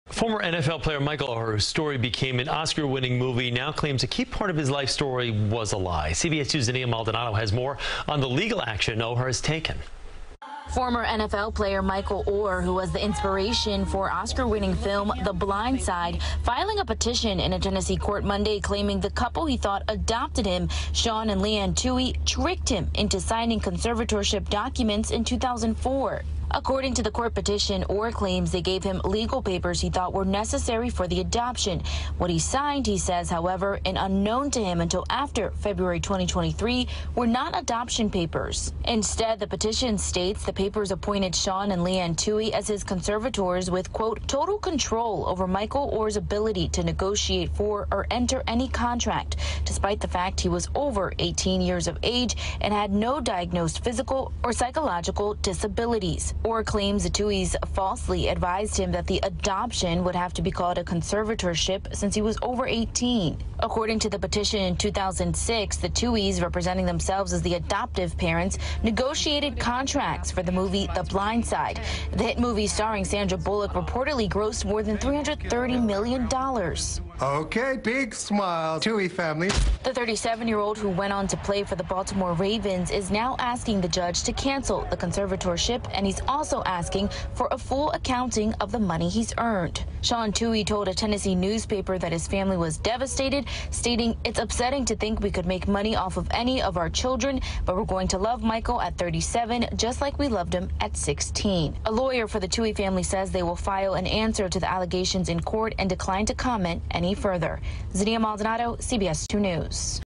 Tuohy family responds to Michael Oher's allegations
Members of the Tuohy family are speaking out after former NFL offensive lineman Michael Oher alleged that they earned millions from pushing a false narrative that they adopted him, the inspiration behind the 2009 Sandra Bullock film "The Blind Side." CBS New York's Zinnia Maldonado reports.